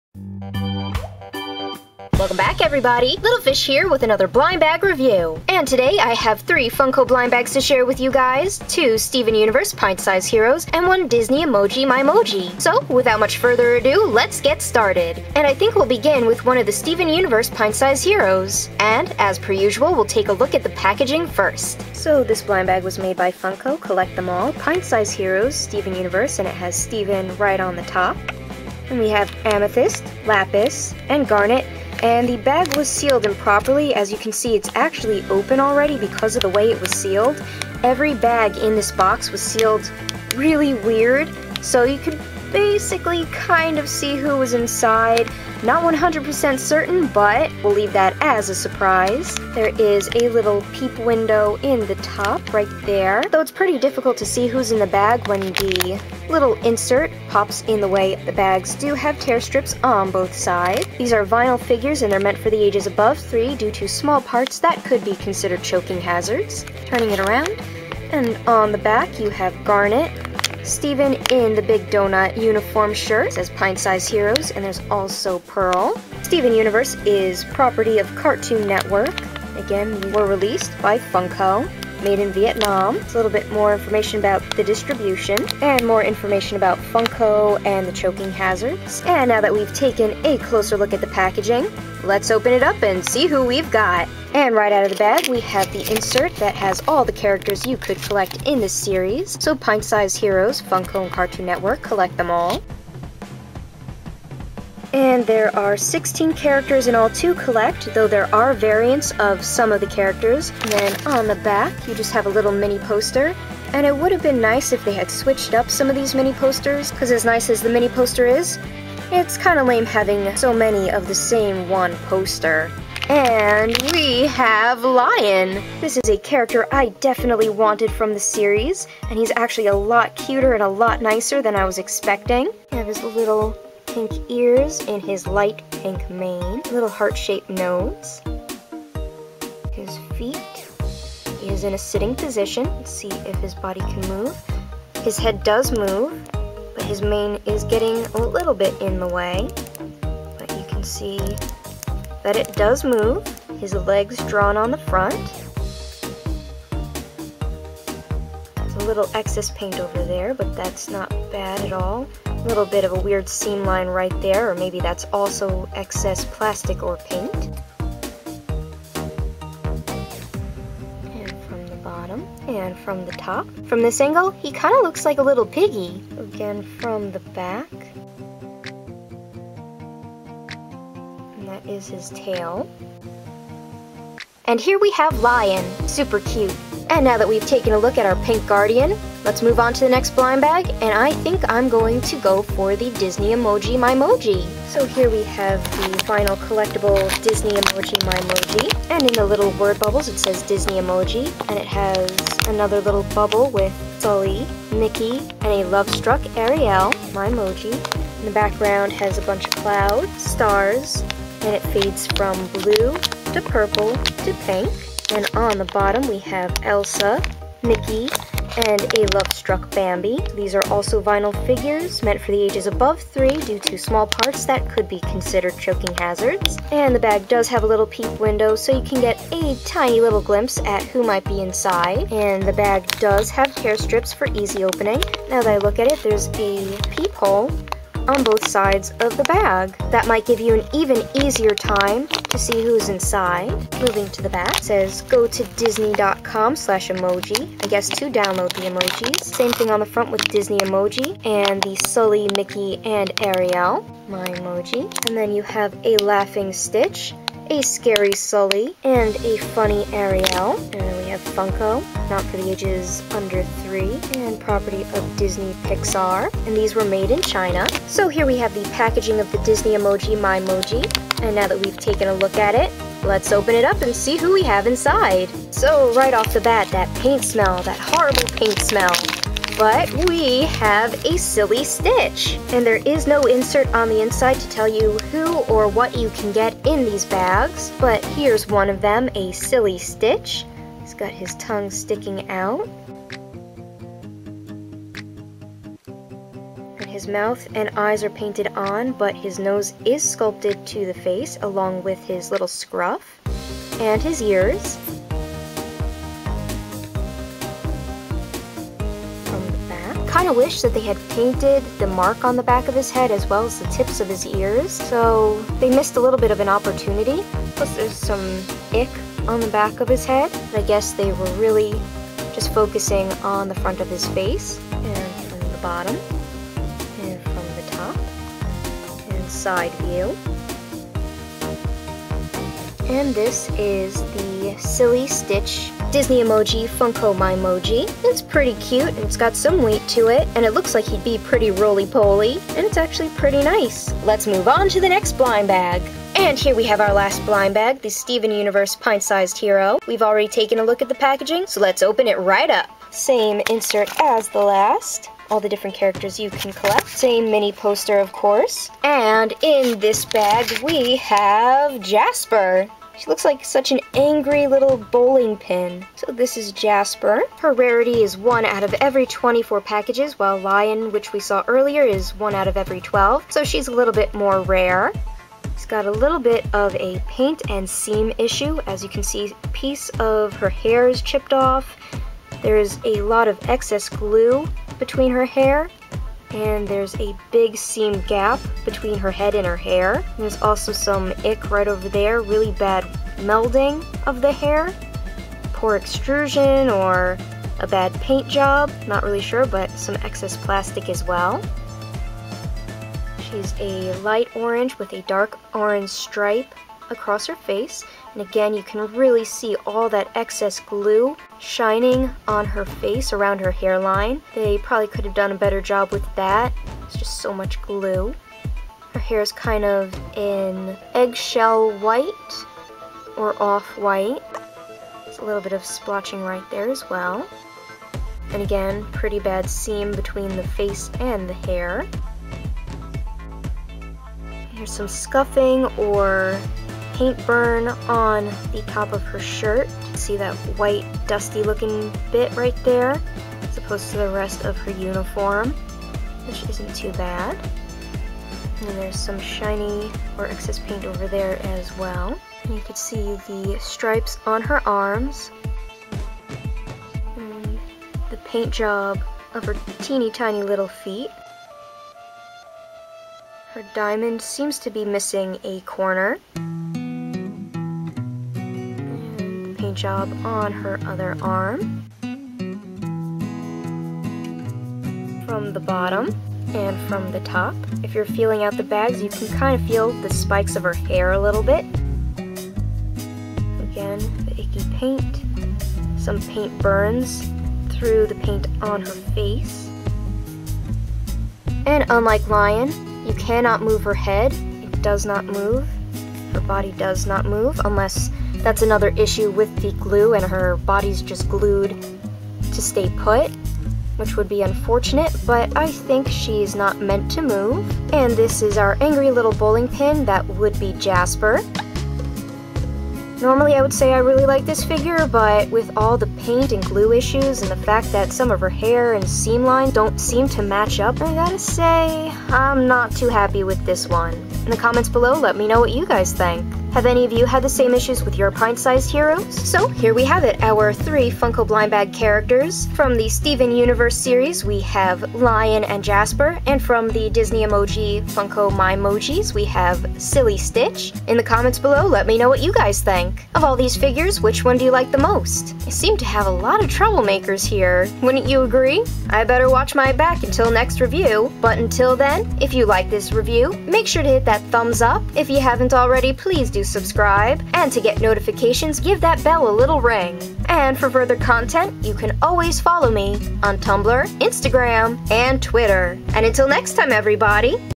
RR 68- Funko Steven Universe Pint Sized Heroes & Disney MyMoji Blind Bags
Today's review features three Funko brand Blind Bags- Two Steven Universe Pint Sized Heroes and a Disney emoji MyMoji, based on the mobile game Disney Emoji Blitz! So let's open them up and see who I get!